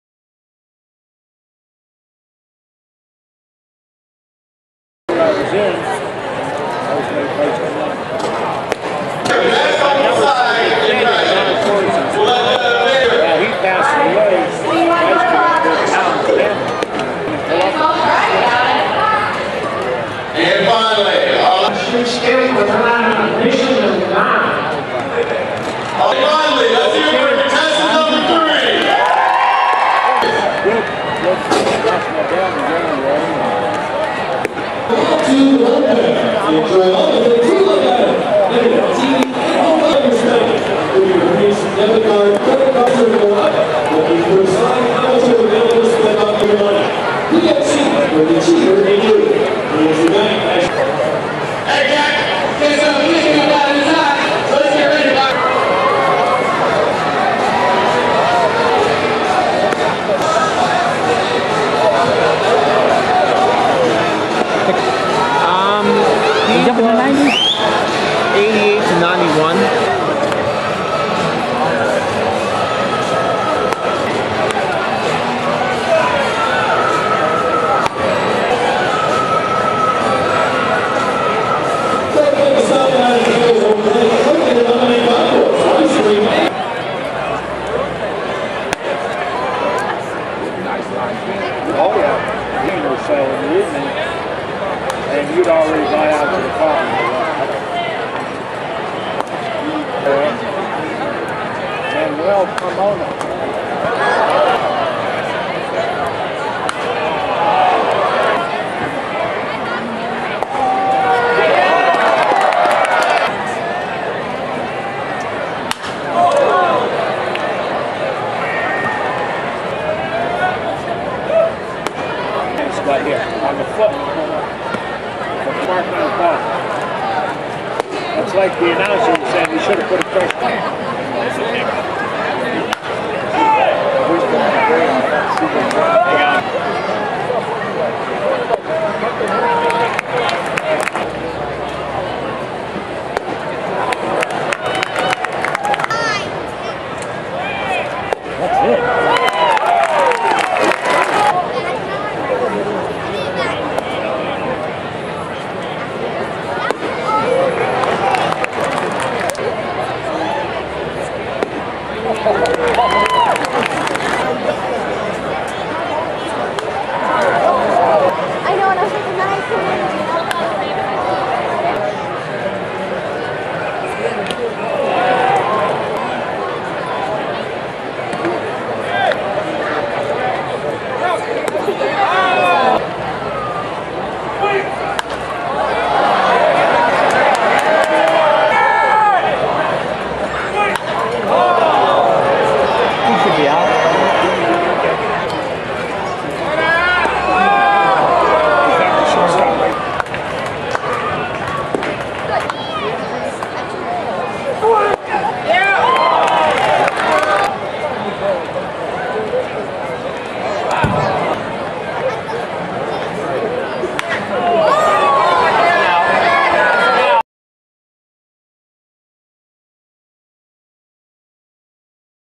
Manuel Carmona - RHP - Mahoning Valley Scrappers (2013-06-27 vs. State College)
Cleveland Indians RHP prospect, Manuel Carmona, vs. State College on June 27th, 2013.